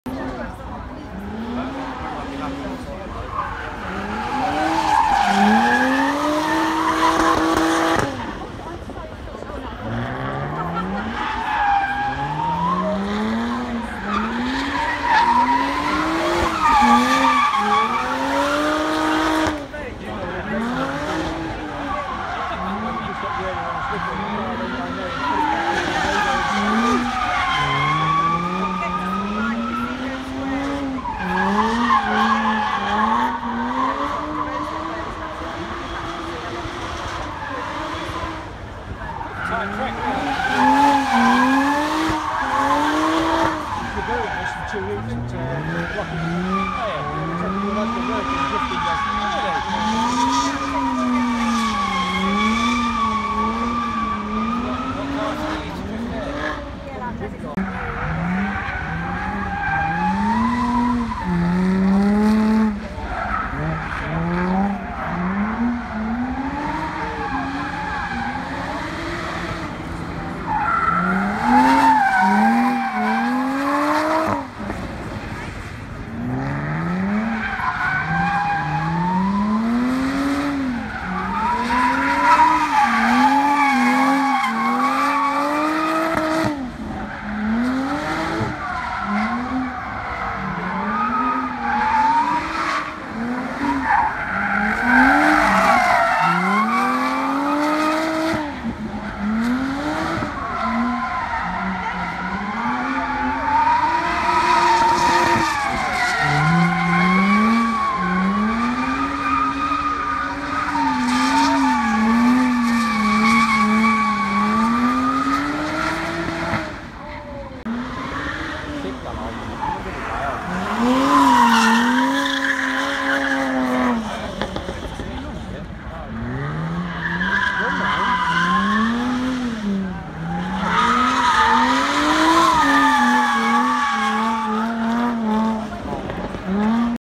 Jaguar F-Types shredding tyres
Goodwood FOS 2018 - Saturday